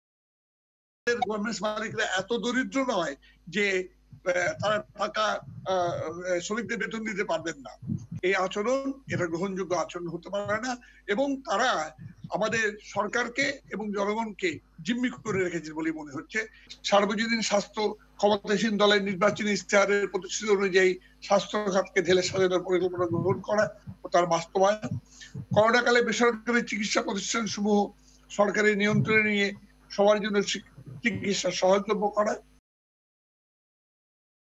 'গার্মেন্টস মালিকরা সরকার ও জনগনকে জিম্মি করে রেখেছে' | Rashed Khan Menon
গার্মেন্টস মালিকরা সরকার ও জনগনকে জিম্মি করে রেখেছে বলে মন্তব্য করেছেন বাংলাদেশের ওয়াকার্স পার্টির সভাপতি রাশেদ খান মেনন। সকালে এক ভিডিও বার্তায় তিনি এসব কথা বলেন। এসময় কোভিড-১৯ মোকাবেলায় স্বাস্থ্যমন্ত্রণালয়ে থোক বরাদ্দ ১০ হাজার কোটি টাকা দুর্নীতির খপ্পরে পড়েছে বলেও সমালোচনা করেন তিনি।

Rashed Khan Menon, president of the Walkers Party of Bangladesh, has said that the garment owners are holding the government and the people hostage. He said this in a video message in the morning. He also criticized that the bulk allocation of Tk 10,000 crore in the Ministry of Health to deal with Kovid-19 has fallen into the clutches of corruption.